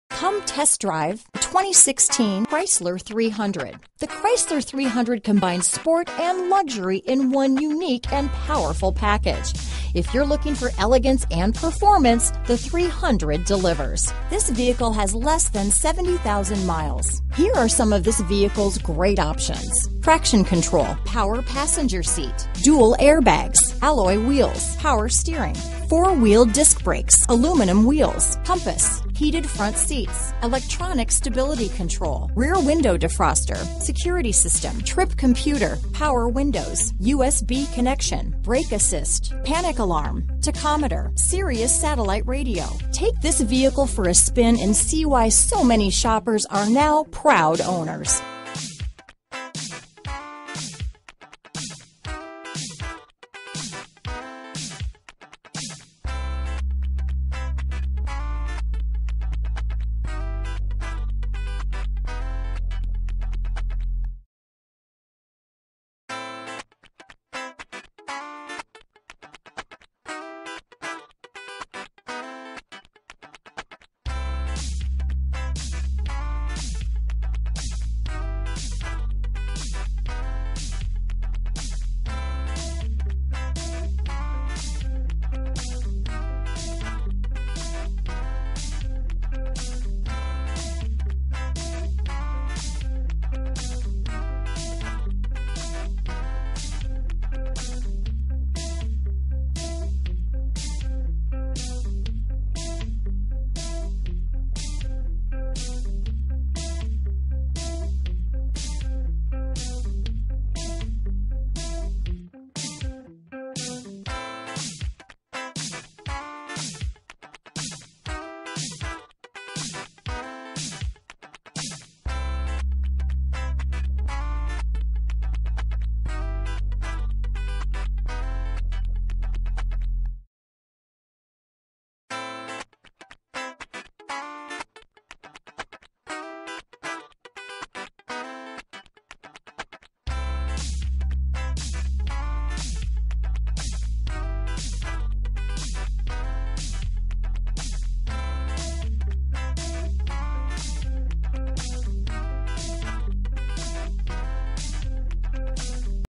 2016 Chrysler 300 El Paso, Las Cruces, Alamogordo, Ruidoso, Carlsbad NA182793A
Billet Silver Metallic Clearcoat Used 2016 Chrysler 300 available in El Paso, Texas at Hyundai of El Paso. Servicing the Las Cruces, Alamogordo, Ruidoso, Carlsbad, TX area.

2016 Chrysler 300 Limited Billet Silver Metallic Clearcoat 3.6L V6 24V VVT 8-Speed Automatic RWD Bluetooth, Backup Camera. CARFAX One-Owner. Clean CARFAX. 19/31 City/Highway MPGbrbrAwards:br  * 2016 KBB.com Best Buy Awards Finalist   * 2016 KBB.com Brand Image Awards